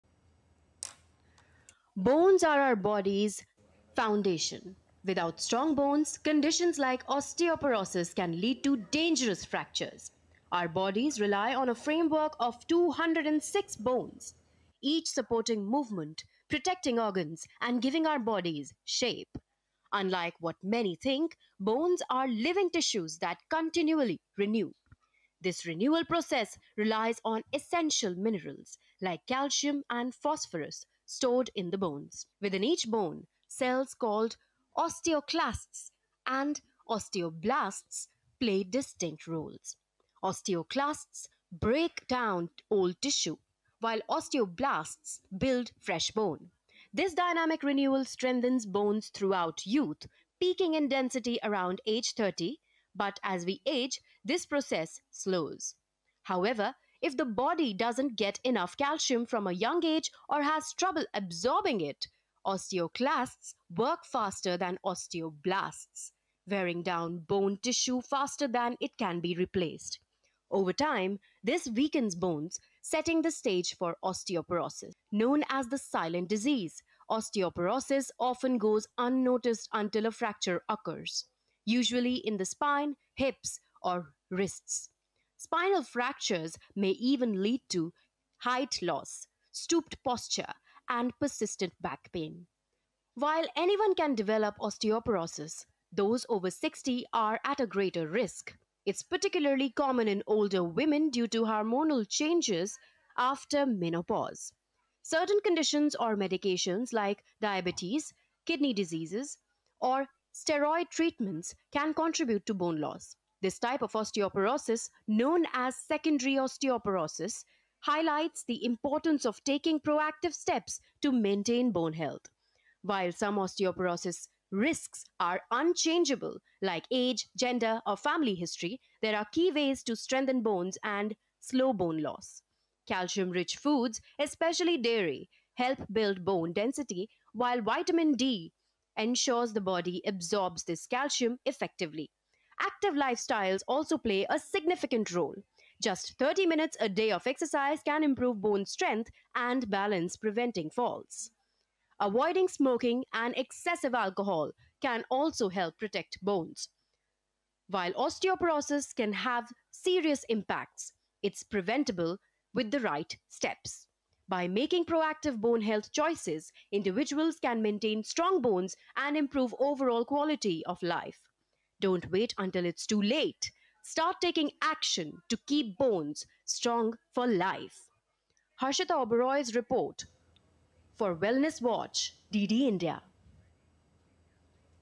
Bones - Human Body's Foundation | Significance & Preventing Osteoporosis | Wellness Watch
Bones form the foundation of our bodies, providing structure, supporting movement, and protecting vital organs. As living tissue, bones continually renew with the help of minerals like calcium and phosphorus, facilitated by bone cells—osteoclasts, which break down old tissue, and osteoblasts, which build new bone. Bone density peaks around age 30 but declines with age, especially if calcium and vitamin D intake are inadequate, leading to conditions like osteoporosis. Known as the "silent disease," osteoporosis often goes undetected until fractures occur, commonly in the spine, hips, or wrists. Risks are higher for those over 60, especially post-menopausal women, and for individuals with certain medical conditions or on specific medications. Maintaining bone health through calcium-rich diets, vitamin D, regular exercise, and avoiding smoking or excessive alcohol can help prevent osteoporosis and enhance overall well-being.